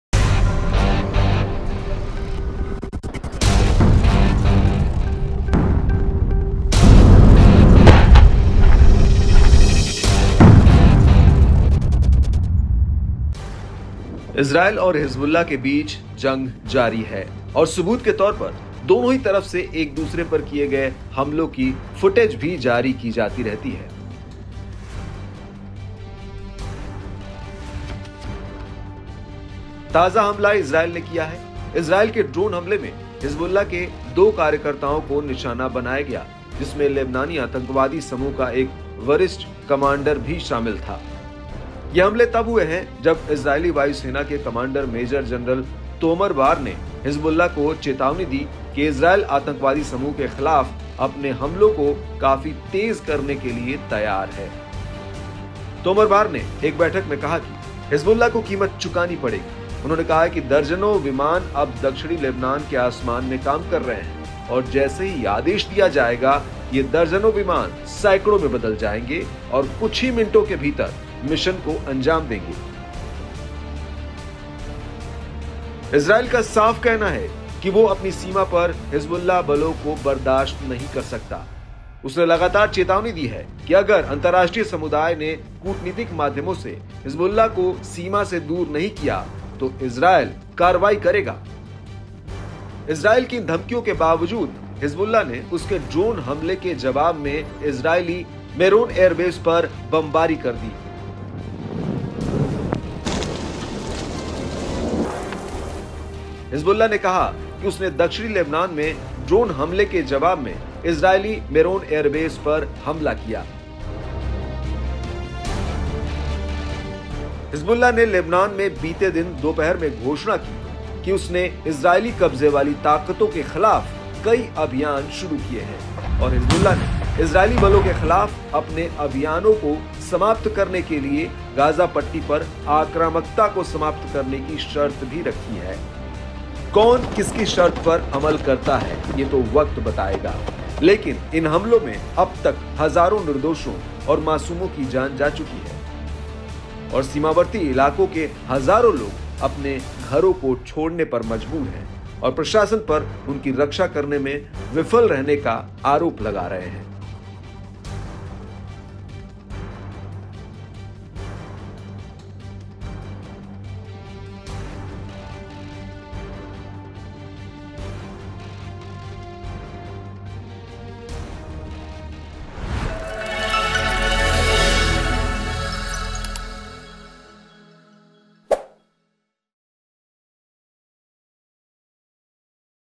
Israel Palestine War : Israel के ड्रोन हमले का Hezbollah ने दिया तगड़ा जवाब | Israel की धमकी बेअसर
The war between Israel and Hezbollah continues and as evidence, footage of attacks carried out by both sides on each other is also released. The latest attack has been carried out by Israel. Israeli drone strike targets 2 Hezbollah operatives, including a senior commander of the Lebanese terrorist group. In response, Hezbollah bombed the Israeli Meron airbase.

इजरायल और हिजबुल्लाह के बीच जंग जारी है और सबूत के तौर पर दोनों ही तरफ से एक-दूसरे पर किए गए हमलों की फुटेज भी जारी की जाती रहती हैं। ताजा हमला इजरायल ने किया है। इजरायल के ड्रोन हमले में हिजबुल्लाह के 2 कार्यकर्ताओं को निशाना बनाया गया, जिसमें लेबनानी आतंकवादी समूह का एक वरिष्ठ कमांडर भी शामिल था। इसके जवाब में हिजबुल्लाह ने इजरायली मेरोन एयरबेस पर बमबारी कर दी।